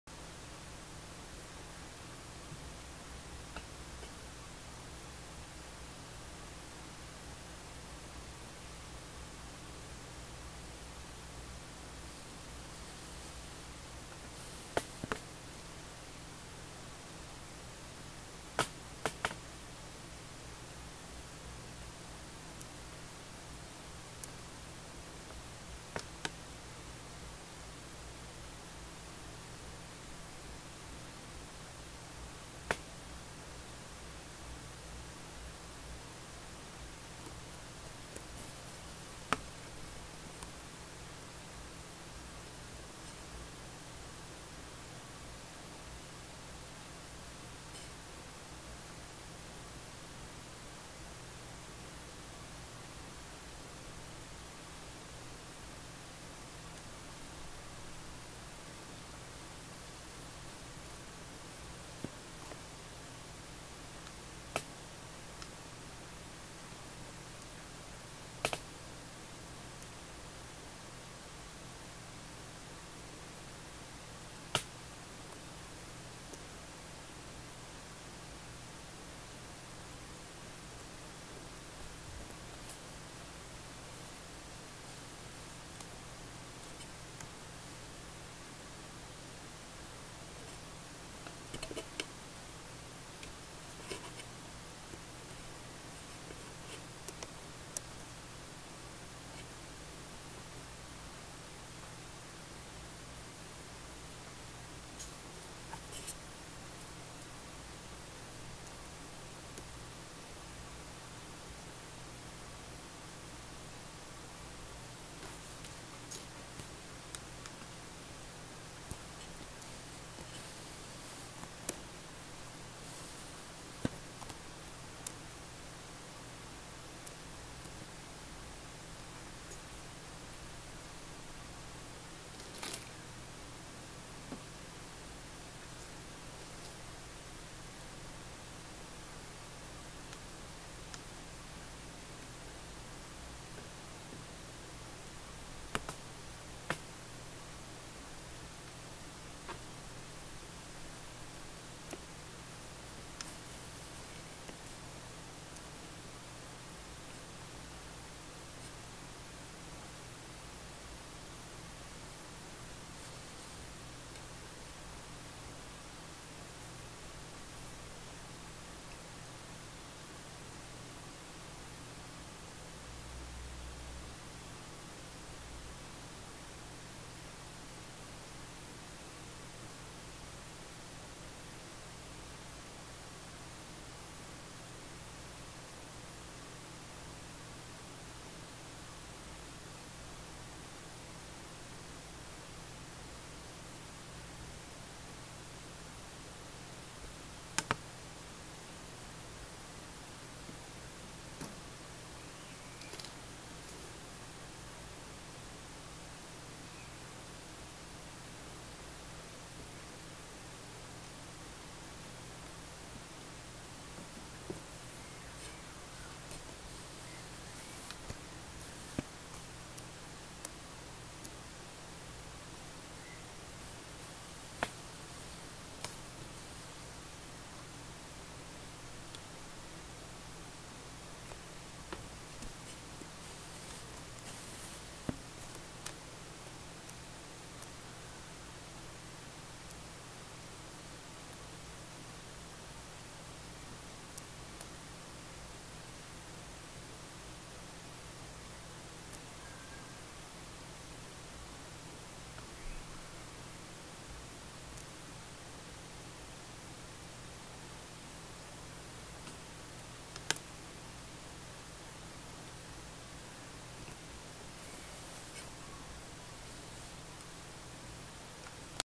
Making [hh/mm] Digital Clock Using Several ICs
NE555 (Timer) * 1
7490 (Counter) * 6
7408 (AND Gate) * 1
7448 (BCD Decoder) * 4
FND500 (7SEGMENT) * 4